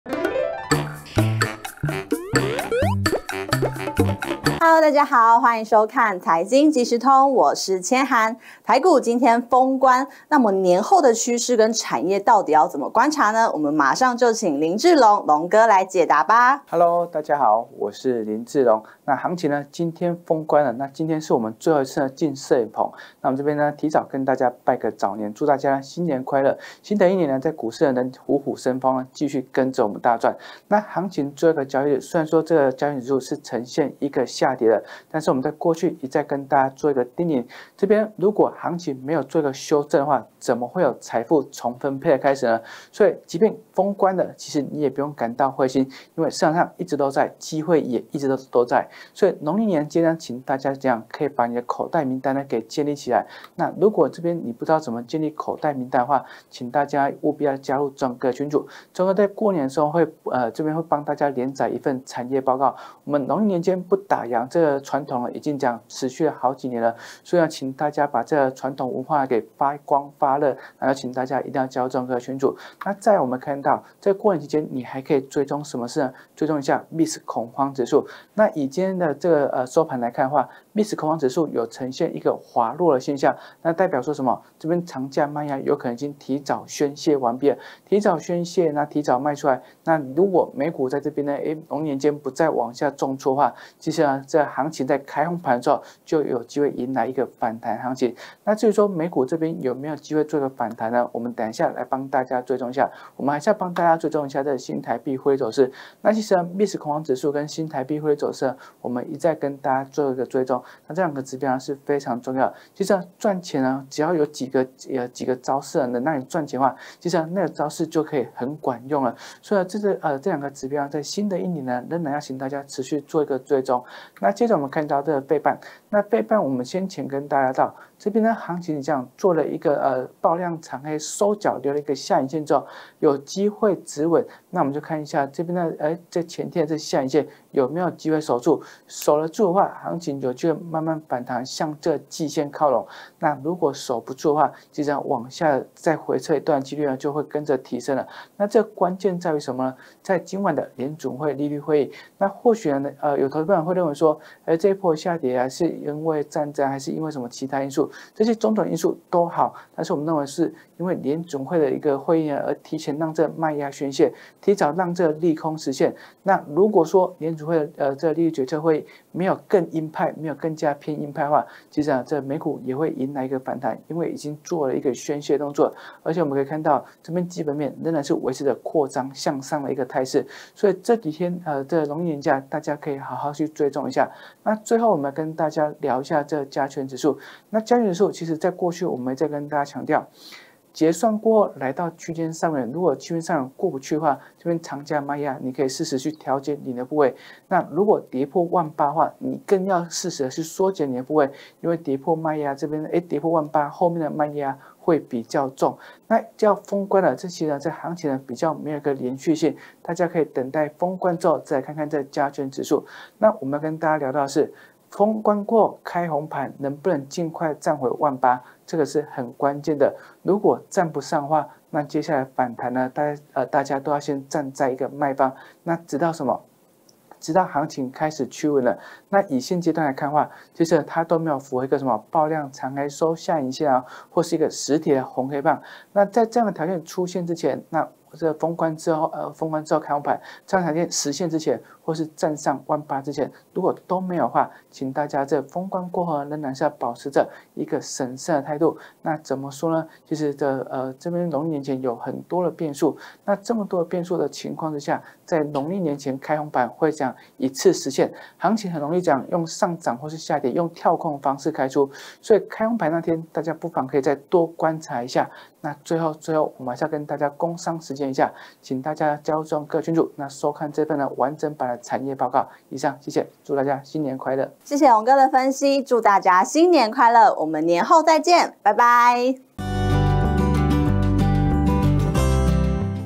財金即時通-20220126／賣壓提早宣洩 年後產業趨勢搶先看！
封關之日台股多空激戰
長假賣壓提早宣洩了嗎❓
年後的盤勢又該如何觀察👀
快來聽聽隆哥怎麼說🔥
🔸手刀掌握財經消息【股動人生】Telegram！
🔸逆勢超抗跌 這族群成開春主流股？